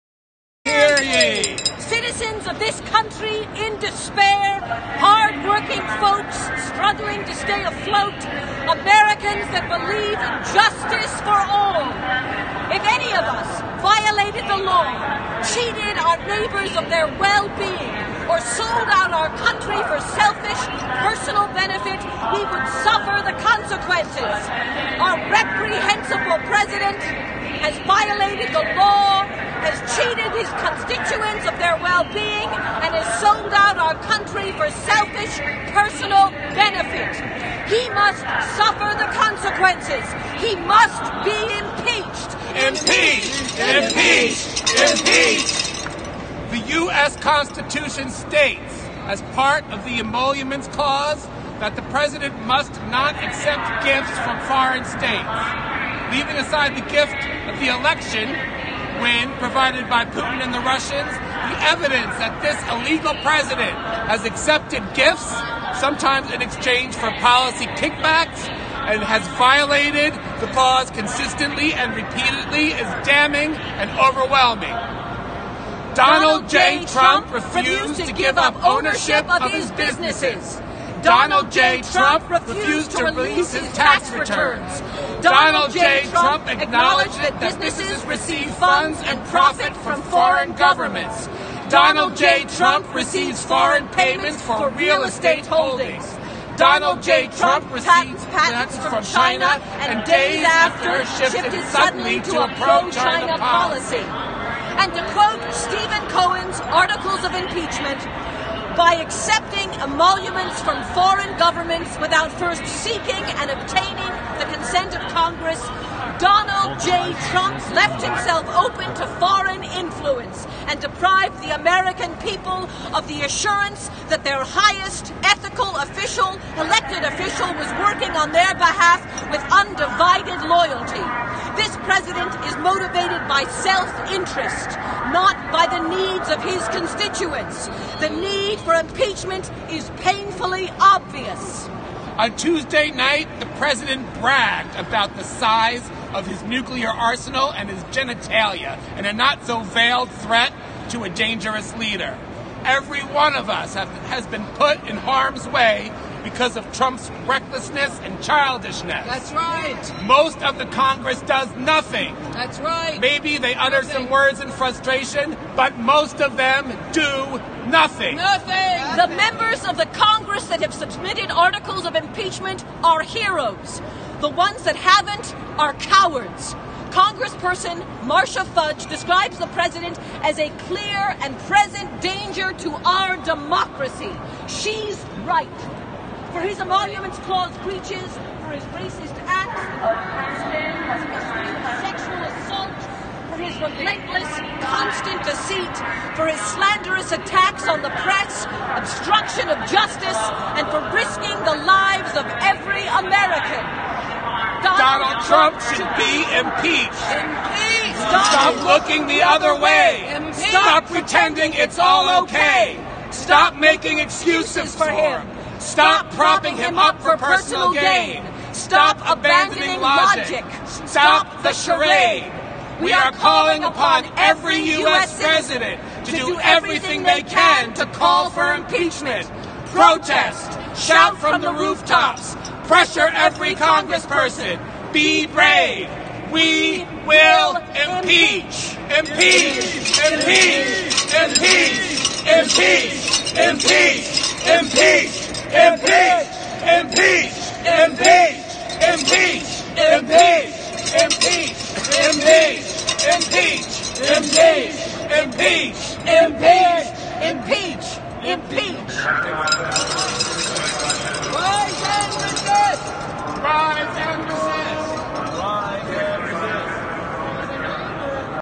RiseNResistNYC 2019 11 28 21 31
Rise and Resist Impeachment protest at Grand Central Station on 1/4/2018. This video was originally recorded live on Twitter with Periscope.